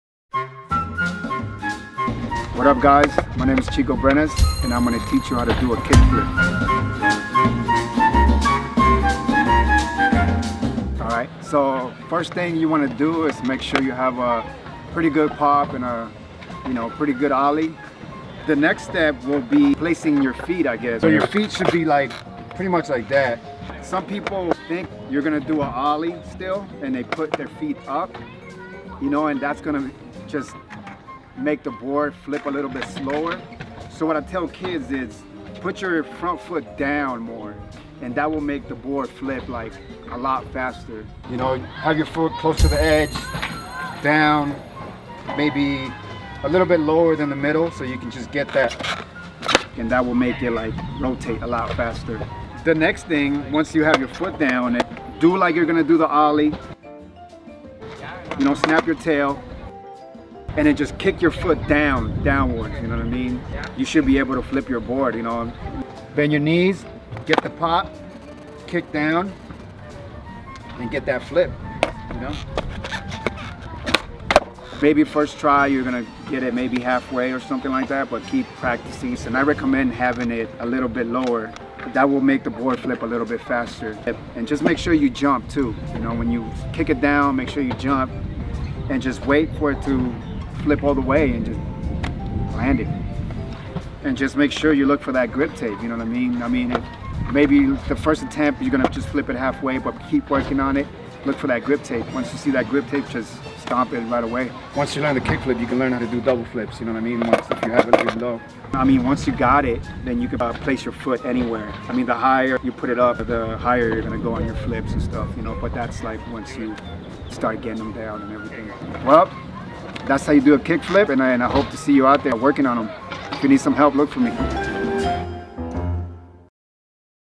Trick Tips: How to Kickflip with Chico Brenes
Chico Brenes, a legend among all skateboarders, lends us some time at Woodward West for this instructional “How To” piece. Chico takes his time to break down the basics of one of the most essential of Skateboard tricks, The Kickflip. From feet placement to height of your pop, Chico explains it in a simple form in order for you to easily understand. Take a look at some of our other “How To” tutorials and make sure you bring it back to camp next summer.

Register For Camp Today!

Camp is always Tweeting and Posting to Instagram and Facebook!